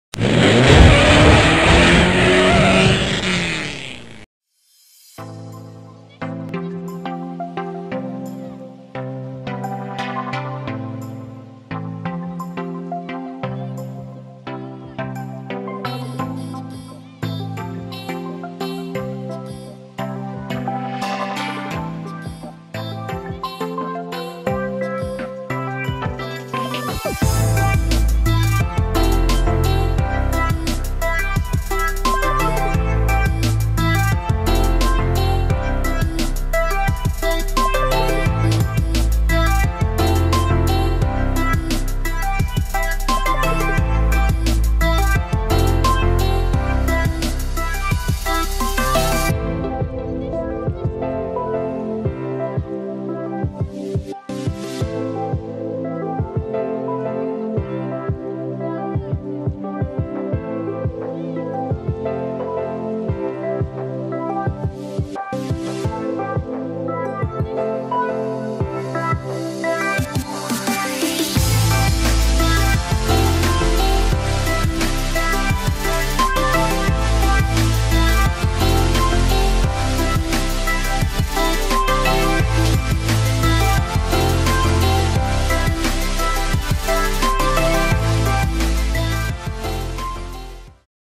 [Showroom] Chevrolet Corvette Fast and Furious diecast car
I am presenting you the “1998 Chevrolet Corvette” from “2 Fast 2 Furious” in this video. It’s not an official movie model. It was made by Ertl in 1/18 scale and it’s limited to 835 pieces.

You can also visit me on facebook and check the pictures of my collection